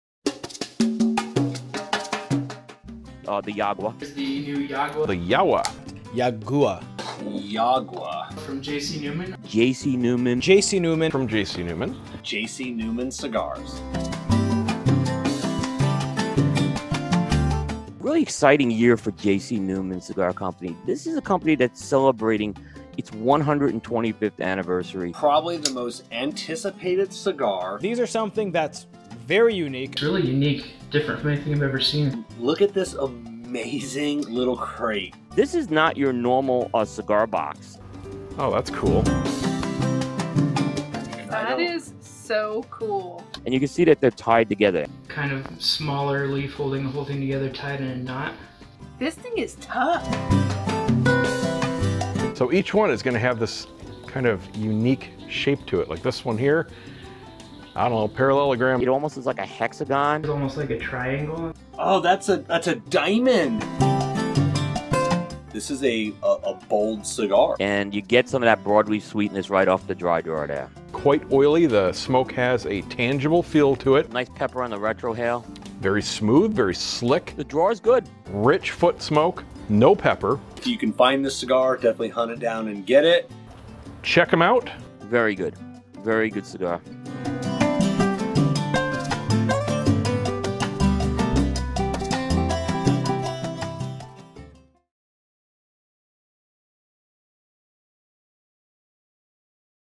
Yagua Unboxing
Many of our great advertising partners took the time to film unboxing videos of the newest cigar from J.C. Newman: Yagua! We took the best pieces and put them together in a compilation video for you to enjoy!

You can find Yagua at a retailer near you by visiting

Cigar Coop
Blind Man's Puff
Cigar Obsession
Cigar Talk with Pats
Cigar Prop
Cigar Dojo